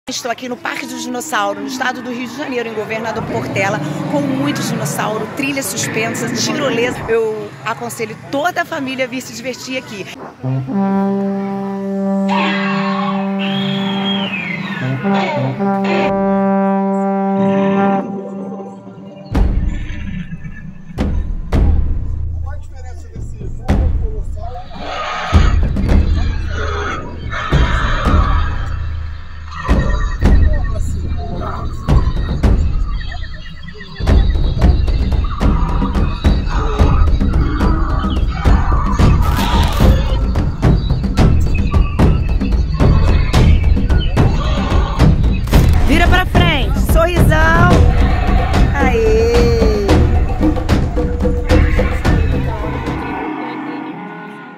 DIA DE AVENTURA NO PARQUE DOS DINOS EM MIGUEL PEREIRA
Imagine uma grande floresta que abriga os mais perigosos dinossauros e as mais variadas aventuras. Um passeio surpreendente e encantador, com Tirolesa, Trilha Suspensa e grandes desafios. Bem vindo a Terra dos Dinos! É claro que a Pietra não poderia ficar de fora dessa e, juntamente com seus primos e familiares, encarou de perto as feras mais temidas do Universo: Tiranossauro Rex (T-Rex), Velociraptor, Diplodoco, Tricerátops, Pterodáctilo, Braquiossauro, Argentinossauro, Brontossauro, Espinossauro, Alossauro, Diplodoco, Anquilossauro, entre outros temidos e famintos dinossauros que habitam entre nós na Terra dos Dinos.